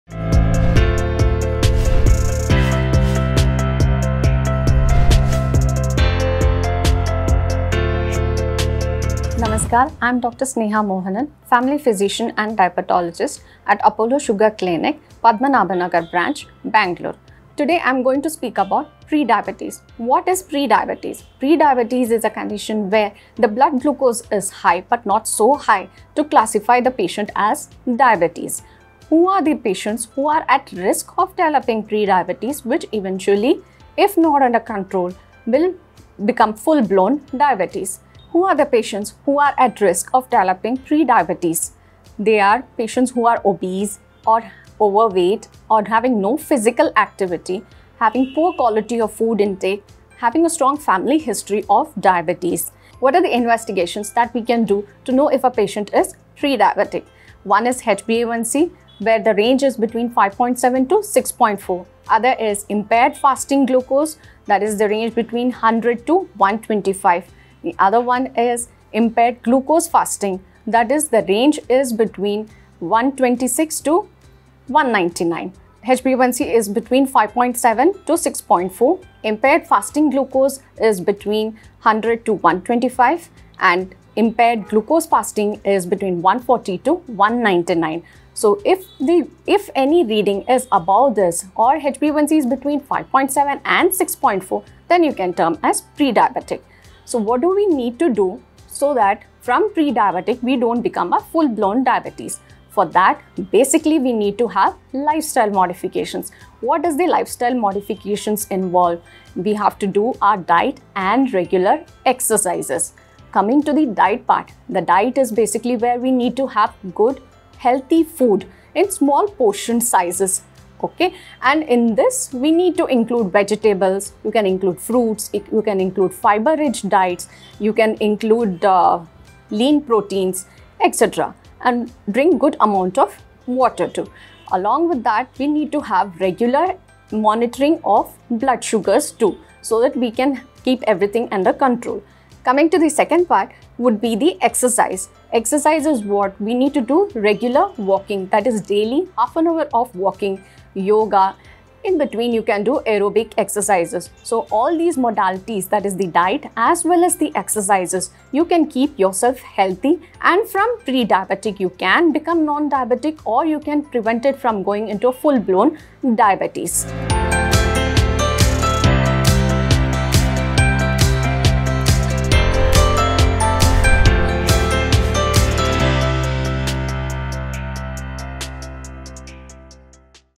Apollo Sugar Clinics | Prediabetes
Watch Dr. Sneha Mohanan, family physician & Diabetologist, Apollo Sugar Clinics, Padmanabha Nagar, Bangalore as she speaks about Prediabetes. Watch the video to know more.